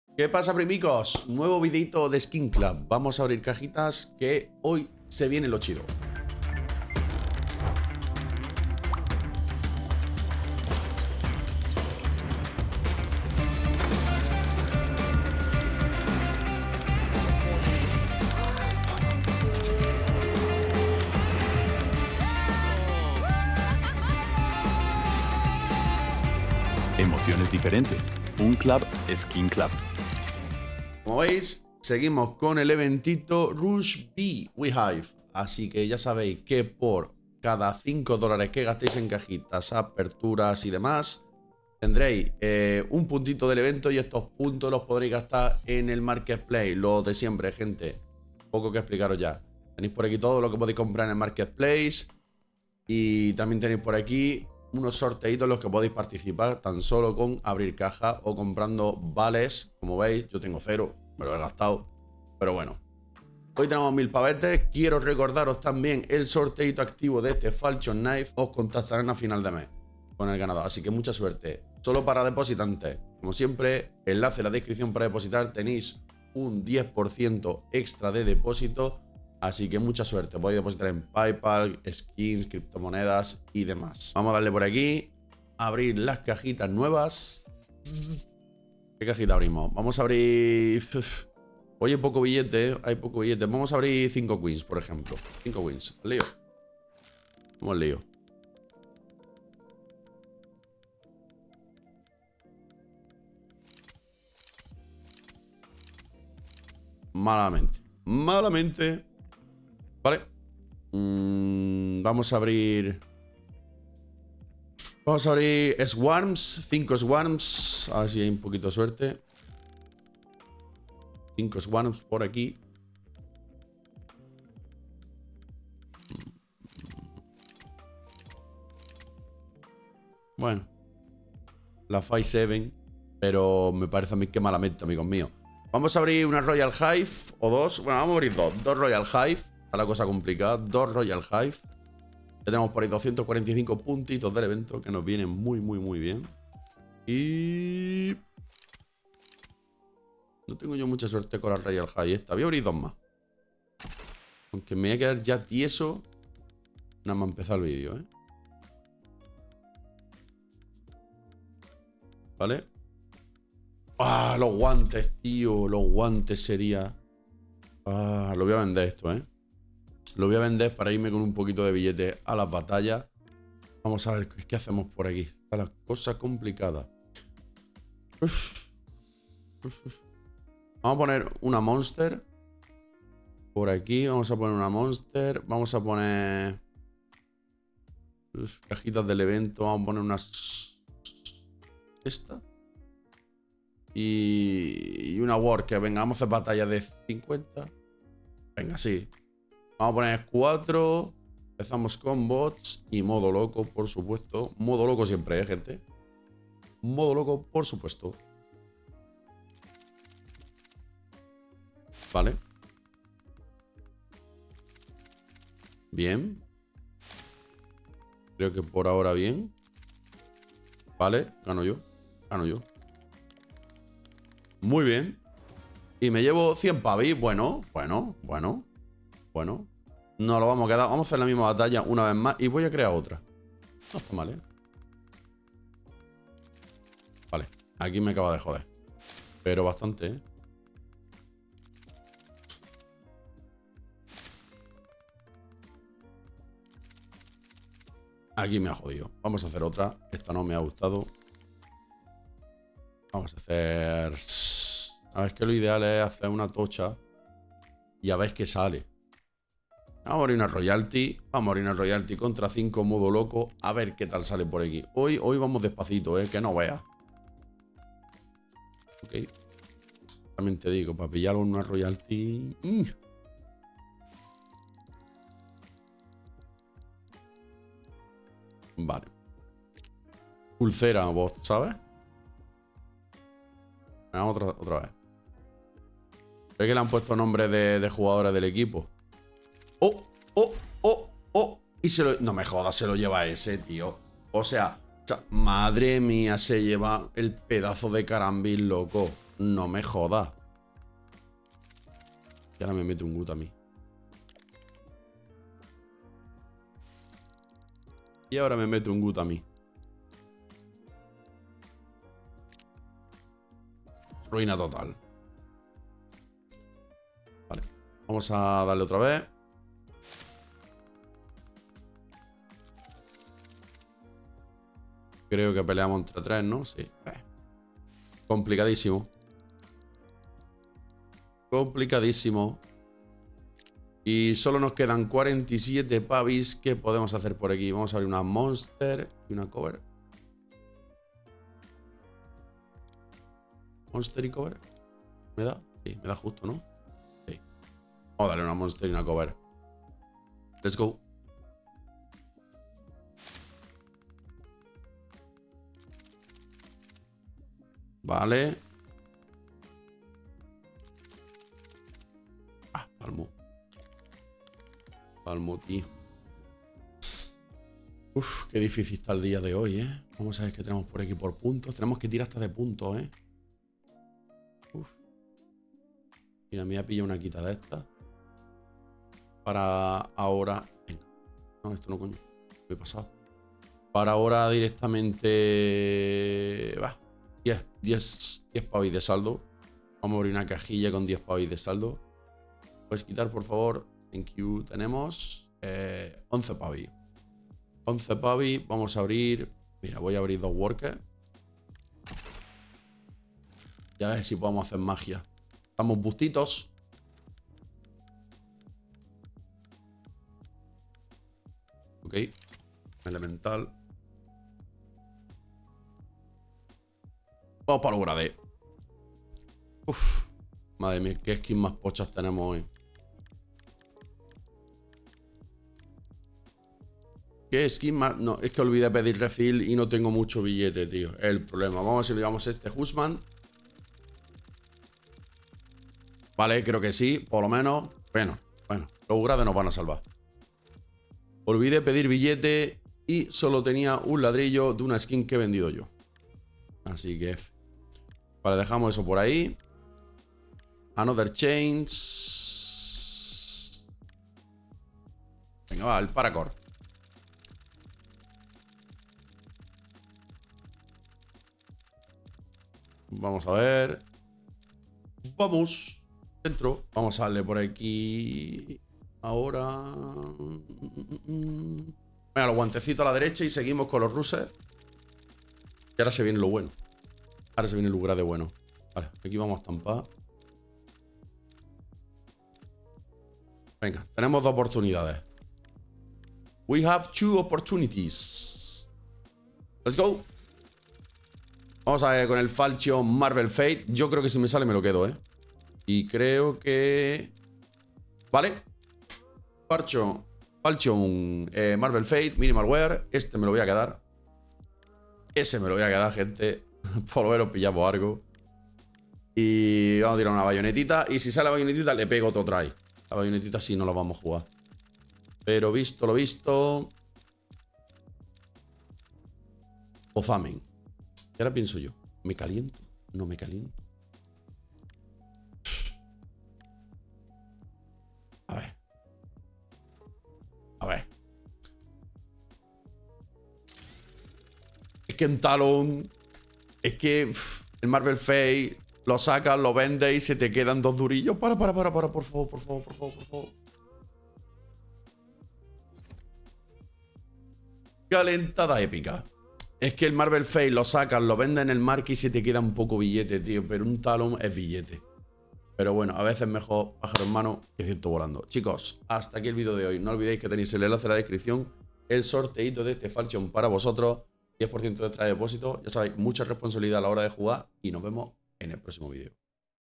ABRO LAS NUEVAS CAJAS de SKINCLUB y lo UPGRADEO TODO! - PROMO CODE 2024 SORTEO!
¿Quieres verme en directo?